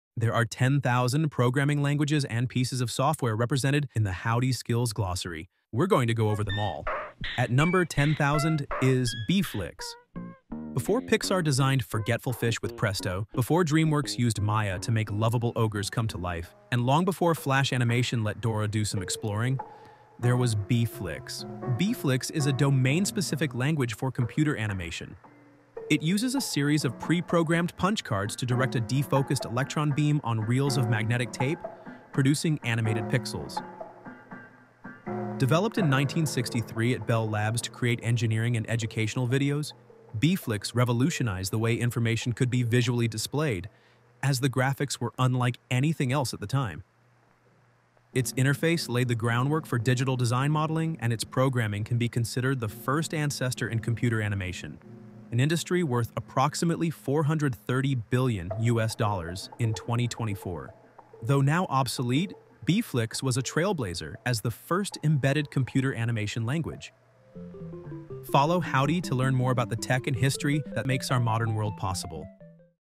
What is Beflix?
Beflix was created in the 1960s by Kenneth Knowlton at Bell Labs. It was developed as an early attempt to explore computer-generated art and graphics, allowing users to create bitmap images through programming. The language was part of a broader effort to investigate the intersection of art and technology.